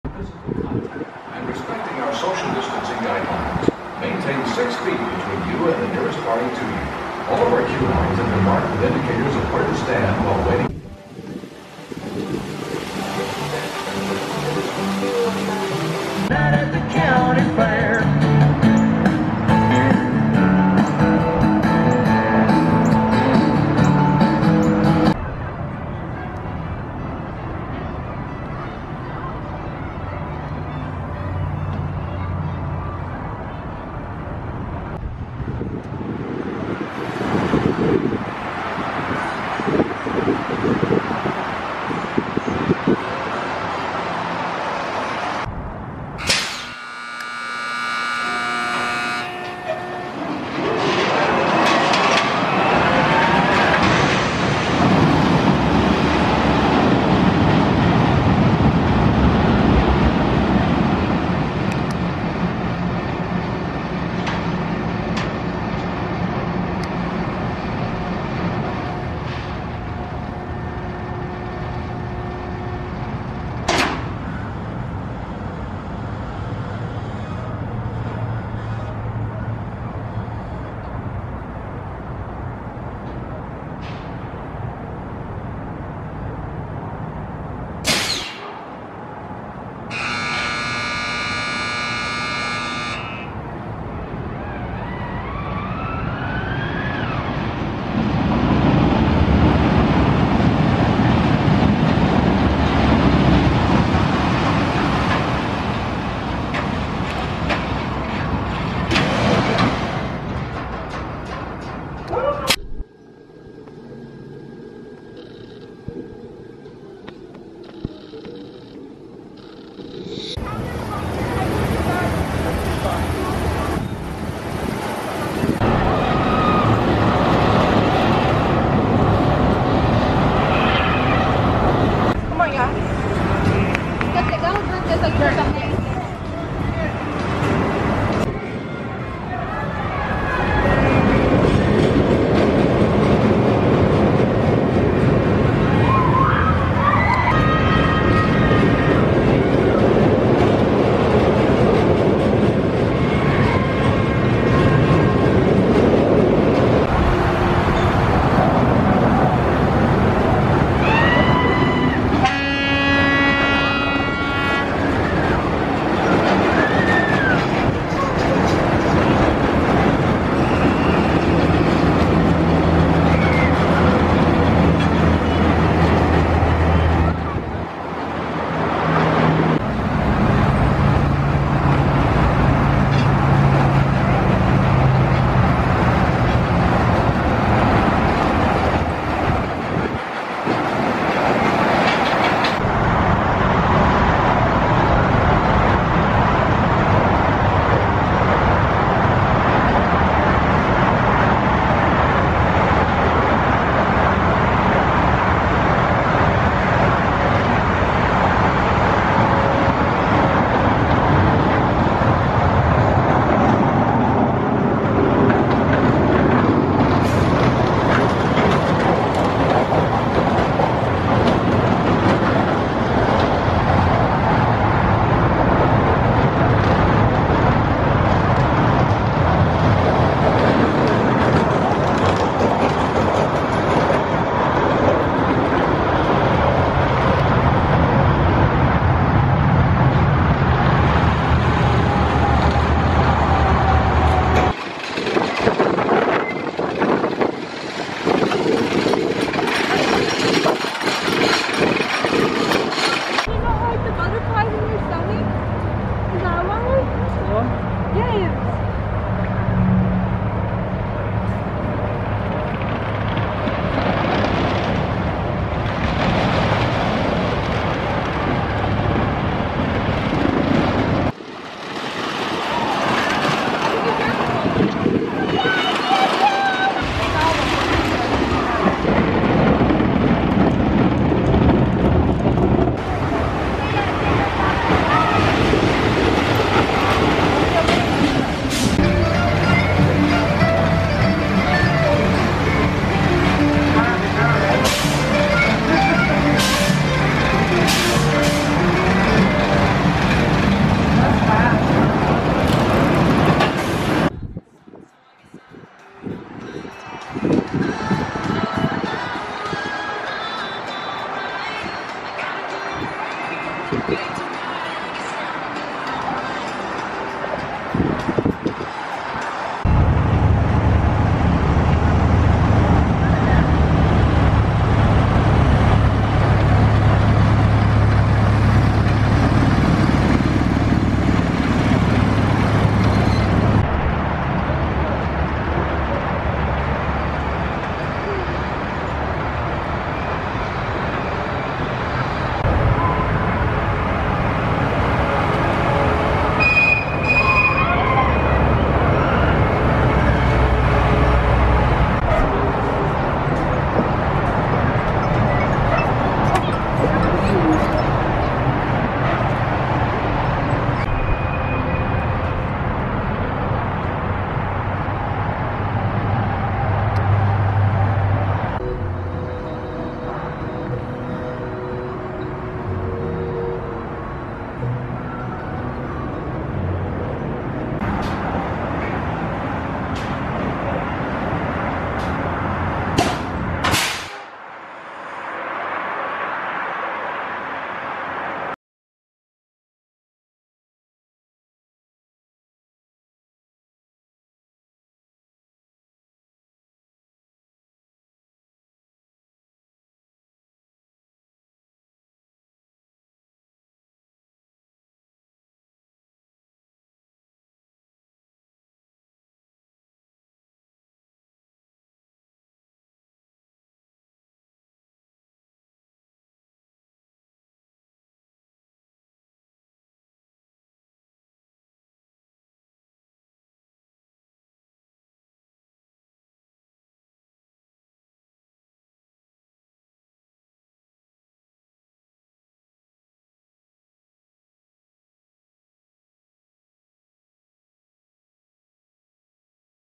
Frontier City Off-Ride Footage, Oklahoma City's Six Flags Park | Non-Copyright
Frontier City in Oklahoma City is one of the best looking Six Flags parks.  Six Flags recently reacquired this western themed park and it includes a handful of rollers coasters like Diamond Back, Silver Bullet, and Wildcat.